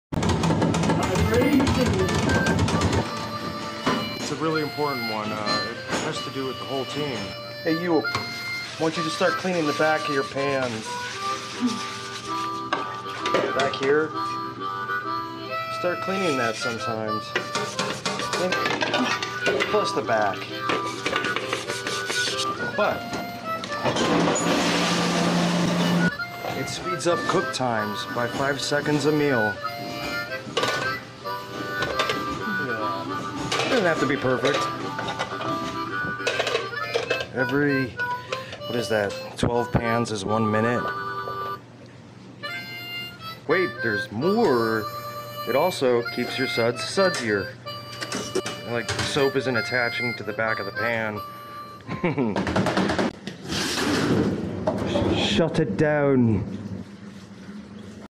Dishwasher's And Line Cooks , listen to this tip... It works .
The best Chefs at the highest end restaurants will demand the back of pans be perfect